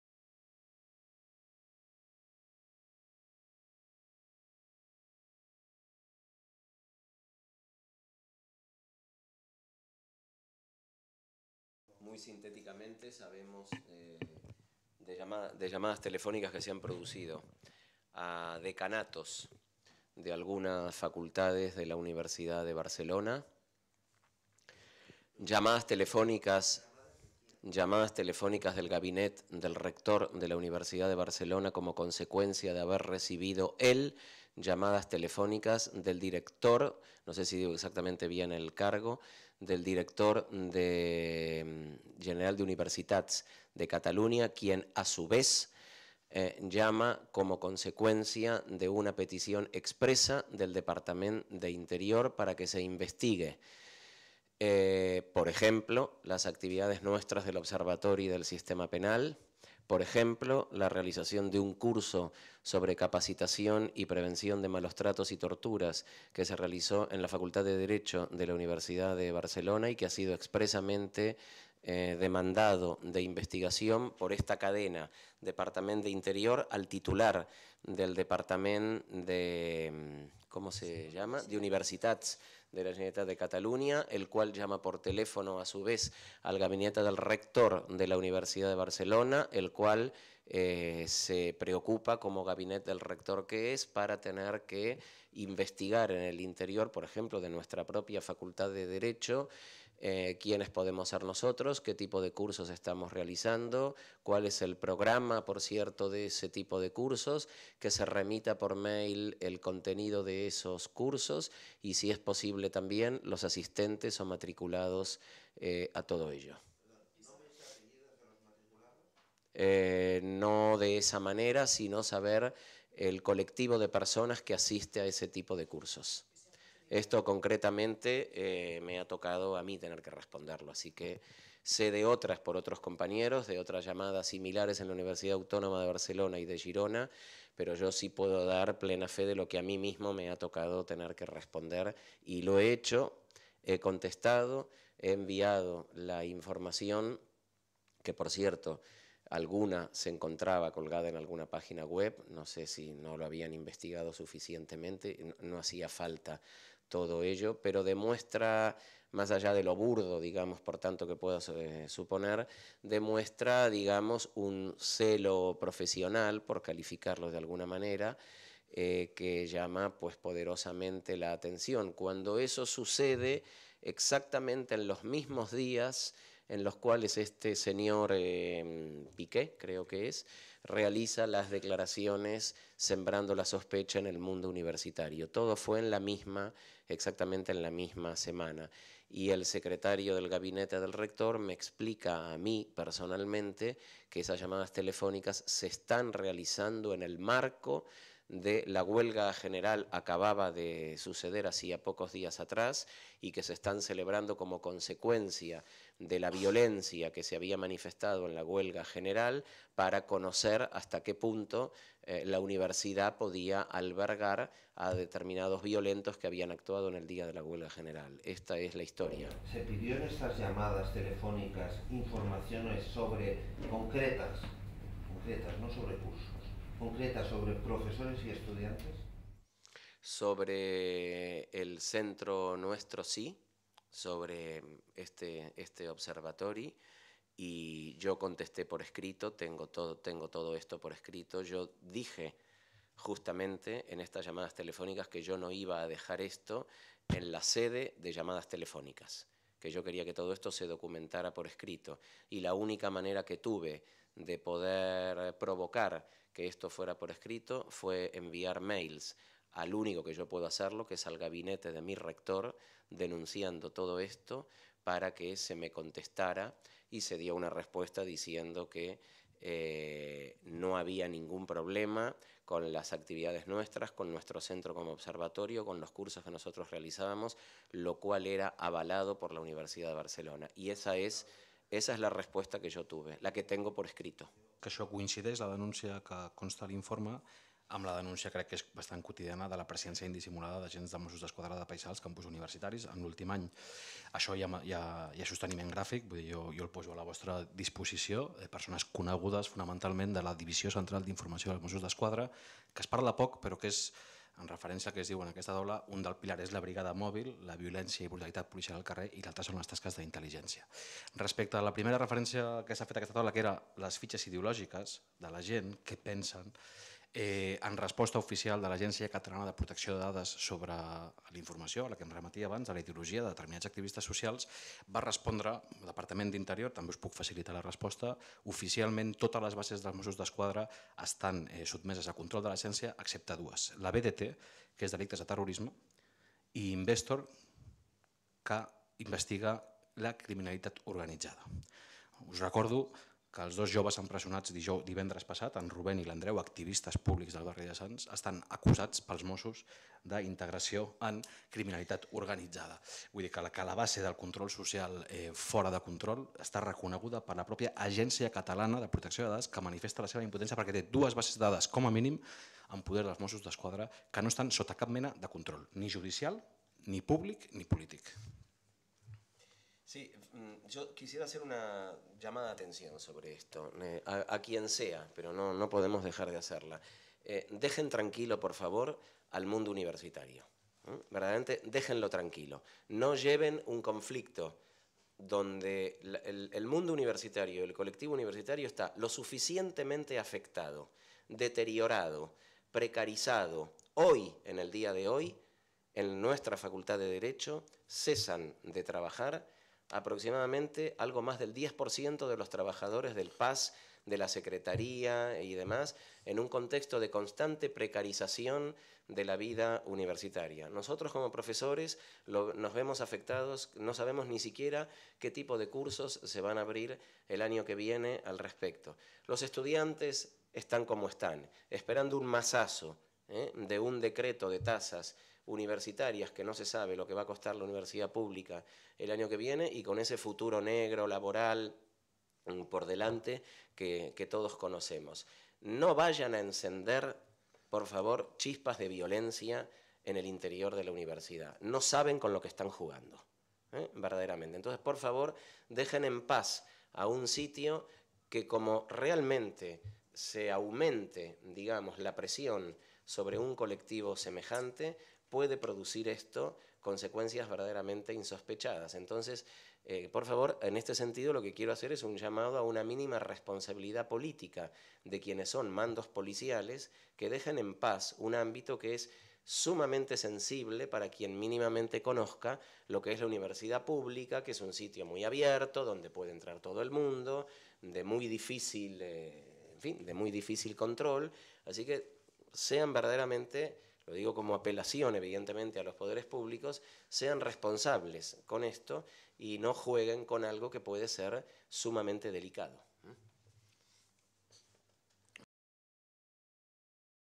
Control policial a les Universitats catalanes (Iñaki Rivera / David Fernández)
6 de juny de 2012. L'Observatori del Sistema Penal i els Drets Humans (UB) presenta l'informe "CRIMINALITZACIÓ DE LA DISSIDÈNCIA, EXPANSIÓ DEL SISTEMA PENAL I SITUACIONS D'ABÚS POLICIAL COM RESPOSTES DAVANT LA CRISI ECONÒMICA A CATALUNYA", davant del COMITÉ EUROPEU PER A LA PREVENCIÓ DE LA TORTURA I ALTRES TRACTES O PENES INHUMANS O DEGRADANTS.